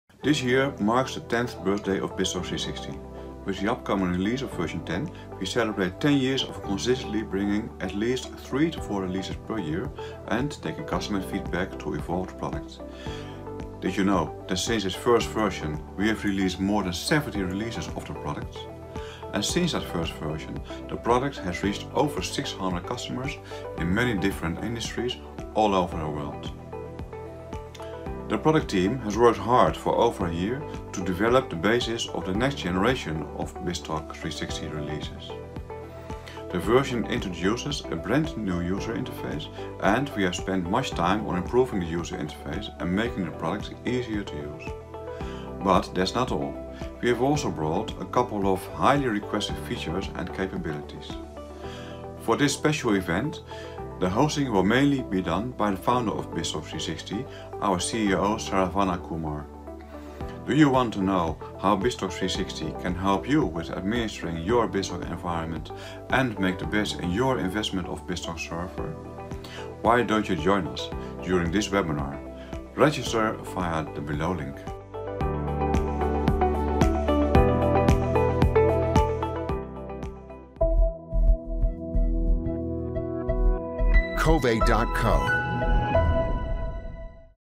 Webinar Teaser - Introducing the All New BizTalk360 v10!
If you are an individual looking for a tool to monitor and administer your BizTalk environment efficiently and securely, then this webinar is for you.

Exactly 10 years ago, BizTalk360 was built with a dream to solve the BizTalk Server monitoring, operations, and analytics challenges in a dynamic fashion.

And, now with 80+ features, 650+ customers across the globe, BizTalk360 has matured to be the #1 tool for BizTalk Server monitoring and administration. We can't be more proud to present version 10 of the product with a whole new collection of features.

So, join us during the webinar on February 25th and listen straight from the visionary of BizTalk360 himself!